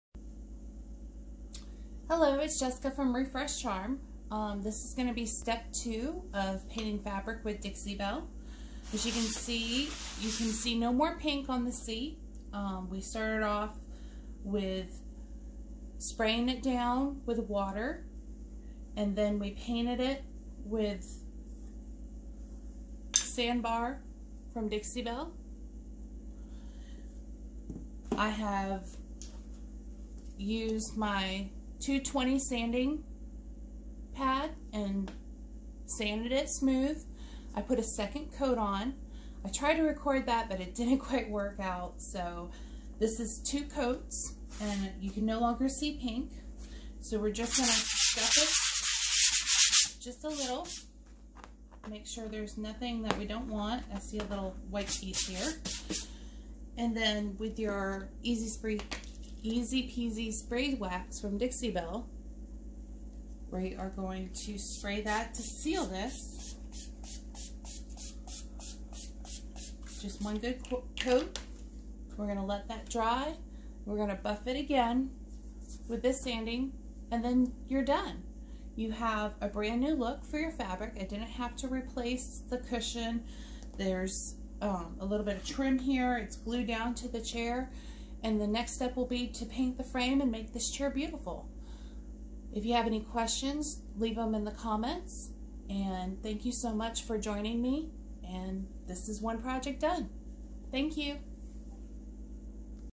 Painting Fabric Step 2
Painting a chair seat with Dixie Belle paint